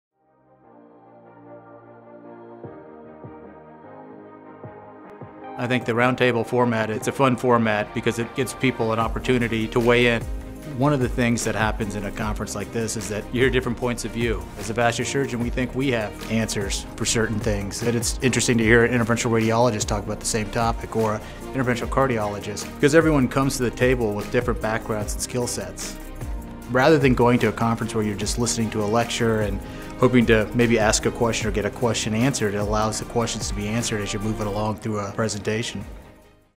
The Roundtable at VIVA
VIVA's signature Roundtable sessions encourage lively debate on current topics and challenges, offering diverse points of view from all areas of vascular medicine and intervention.